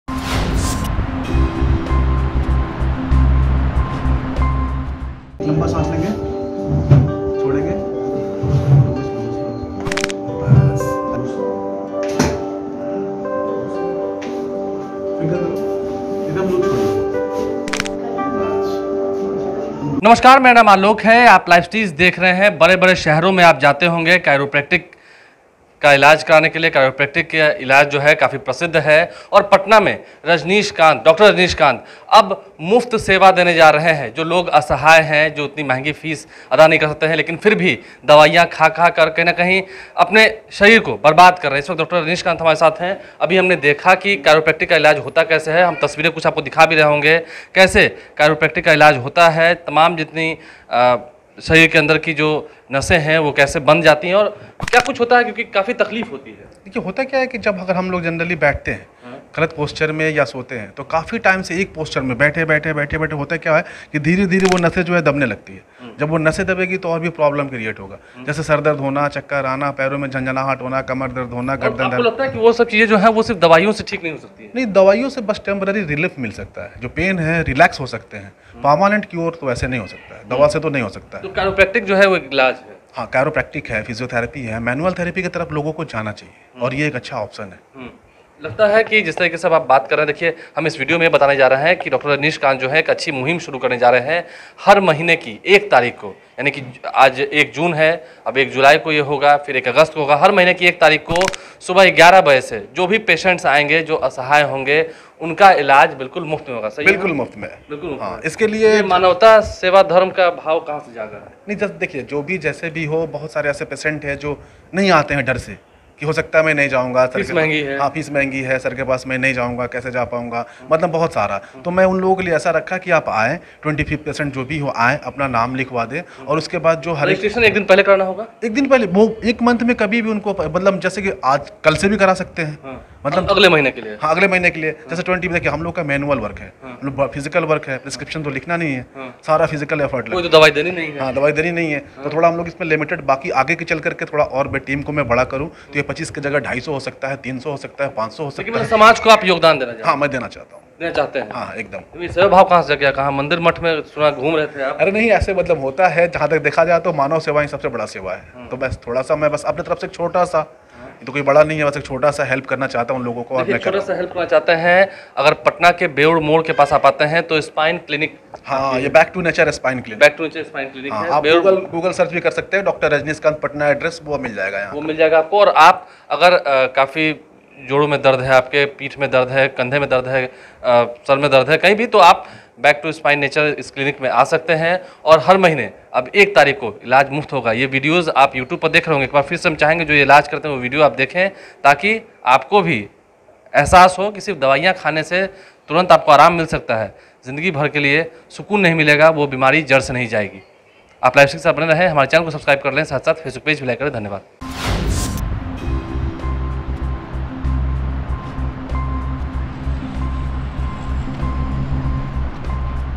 Bihar के Dr. Rajneesh Kant करेंगे गरीबों का मुफ्त में Chiropractic Adjustment with Loud Cracks...
Bihar के Dr. Rajneesh Kant करेंगे गरीबों का मुफ्त में Chiropractic Adjustment with Loud Cracks...| Bihar News | LiveCities